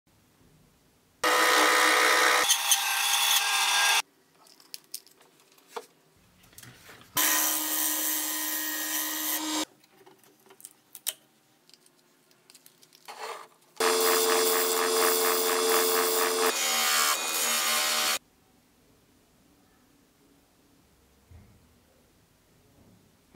Woodturning an off centre pendant on the wood lathe
In this video I show you how to make an off centre pendant on the wood lathe.

 Tools and materials I propose:
Hot glue gun kit
Buffing kit
Heat gun
Chuck kit
Tormek T8 Sharpening System
Tormek Woodturner’s Kit